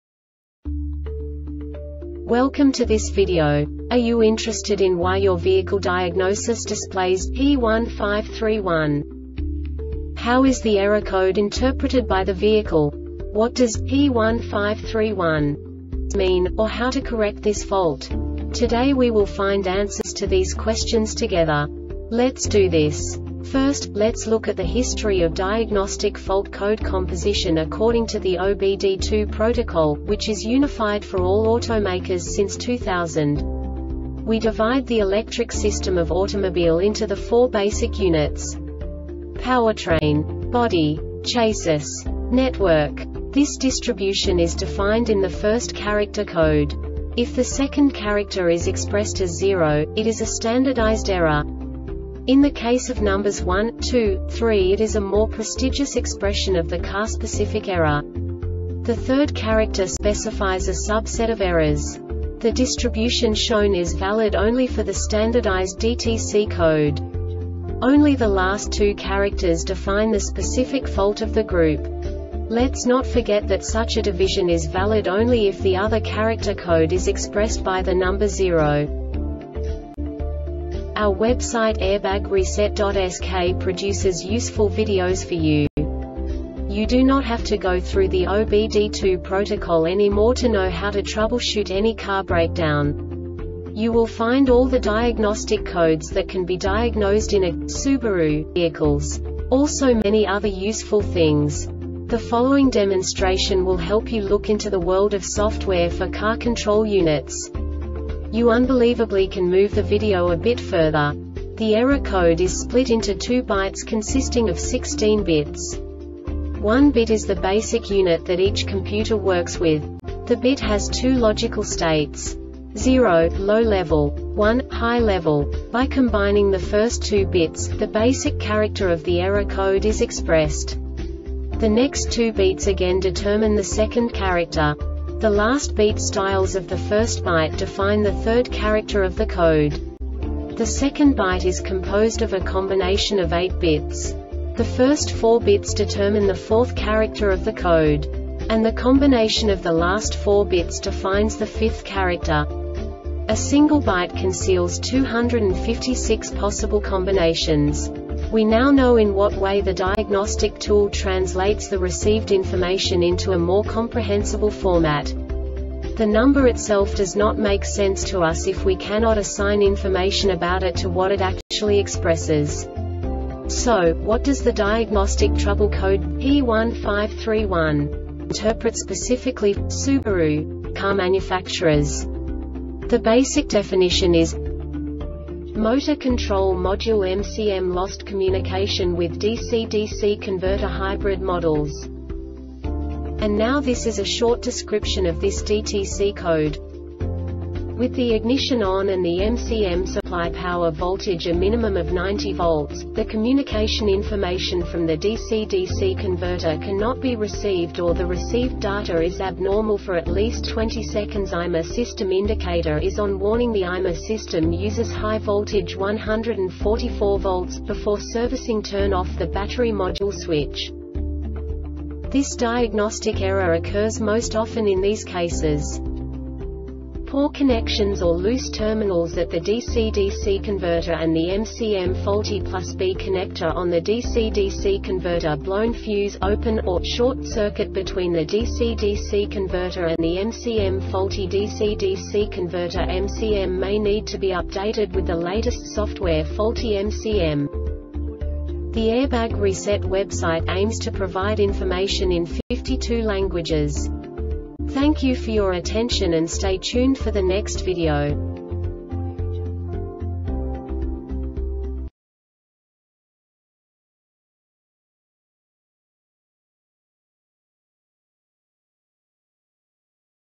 DTC Subaru P1531 Short Explanation
Contents:
0:21 Basic DTC analysis according to OBD2 protocol standard.
1:48 Insight into programming
2:58 A diagnostically understandable description of the Diagnostic Trouble Code
3:05 Basic error definition

Make:
Subaru
Code:
P1531
Definition:
Motor Control Module (MCM) Lost Communication with DC-DC Converter (HYBRID MODELS)
Description:
With the ignition ON and the MCM supply-power voltage a minimum of 9.0 V,the communication information from the DC or DC converter can not be received or the received data is abnormal for at least 2.0 seconds.(IMA system indicator is ON) WARNING: The IMA system uses high voltage (144 V),before servicing turn OFF the battery module switch.
Cause:
 Poor connections or loose terminals at the DC-DC converter and the MCM Faulty +B connector on the DC-DC converter Blown fuse "Open" or "Short" circuit between the DC-DC converter and the MCM Faulty DC-DC converter MCM may need to be updated with the latest software Faulty MCM